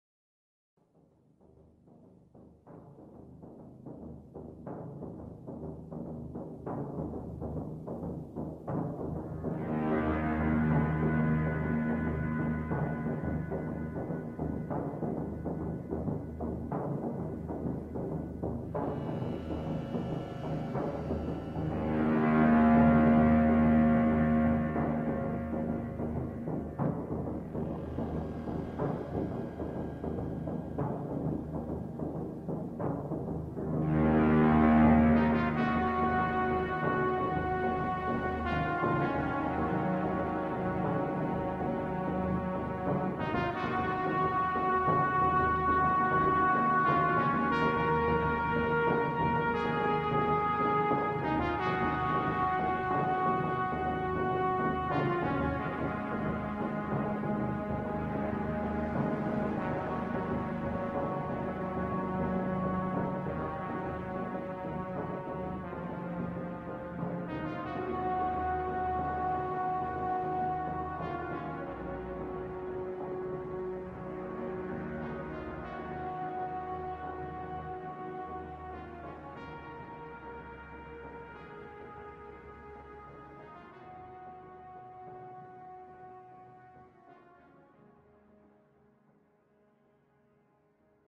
Devils Dyke Sunrise timelapse
03/05/2014, Devils Dyke in Sussex UK.  Sunrise time lapse using 1423 still images..